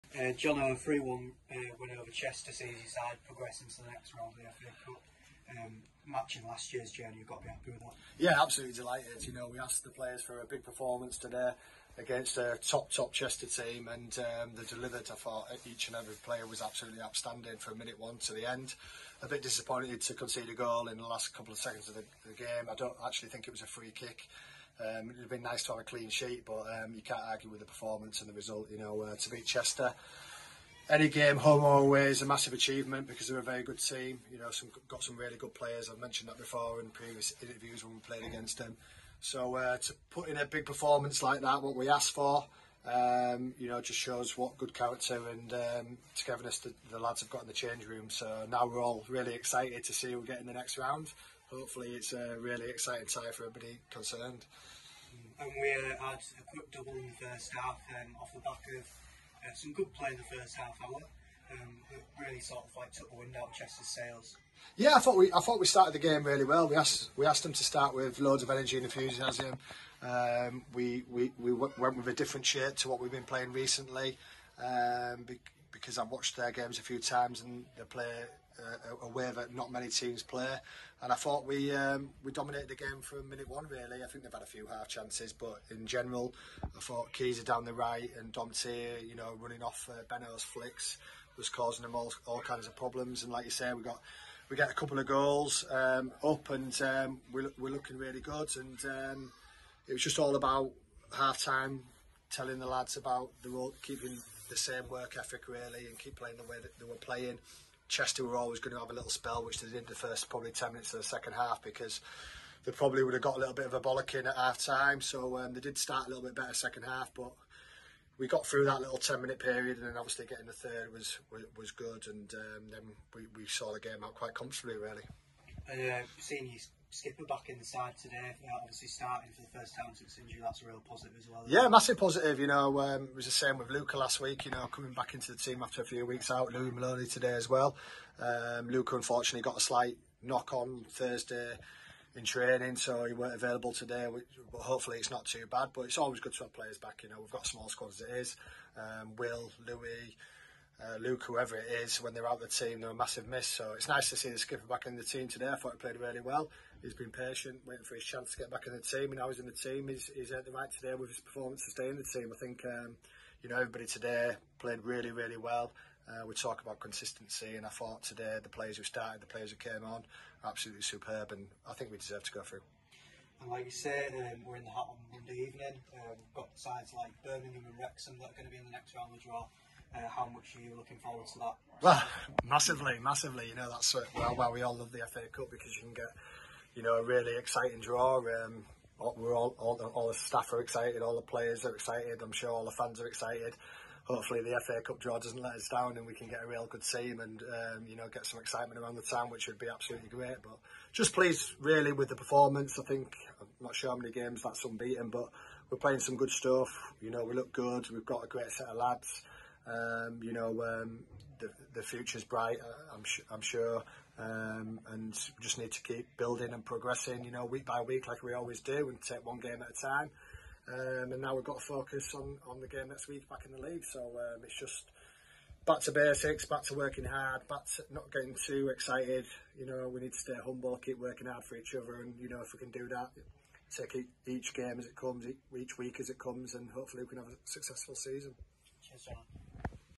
POST MATCH INTERVIEW | Jono Greening Vs Chester FA Cup
The Seadogs progress into the First Round Proper!
Boro breeze through to the next round of the Emirates FA Cup after a 3-1 win against Chester FC.

Next game: Southport at home 19th October

Interview: Ryan Furnell